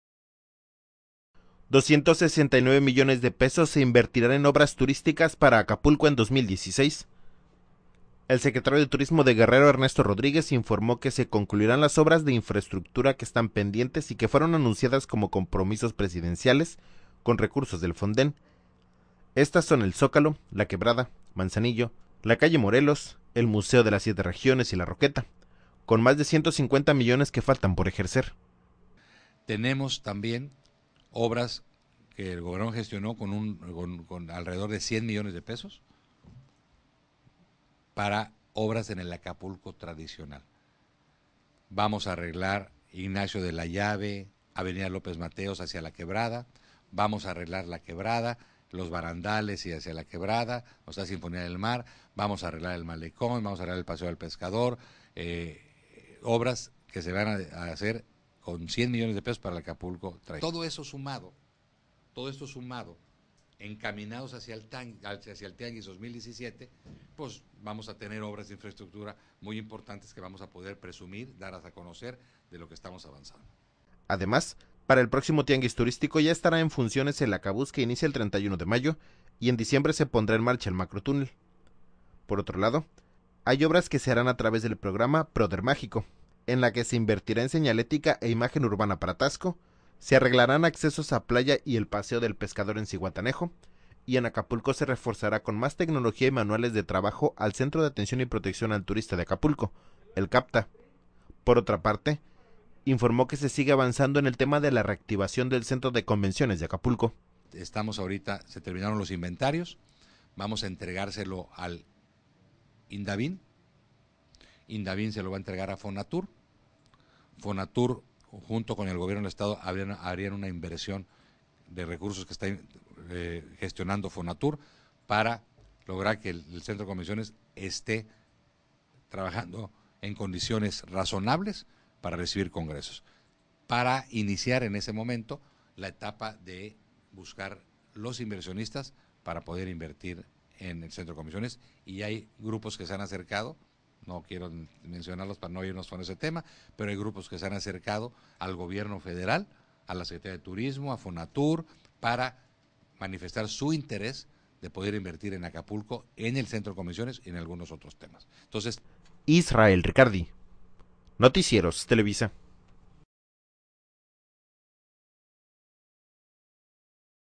269 millones de pesos se invertirá en 2016 para Acapulco en 2016.
El Secretario de Turismo de Guerrero, Ernesto Rodríguez informó que se concluirán las obras de infraestructura que están pendientes y que fueron anunciadas como compromisos presidenciales con recursos del FONDEN. Estas obras son El Zócalo, La Quebrada, Manzanillo, la calle Morelos, el Museo de las 7 Regiones y La Roqueta, con más de 150 millones que faltan por ejercer.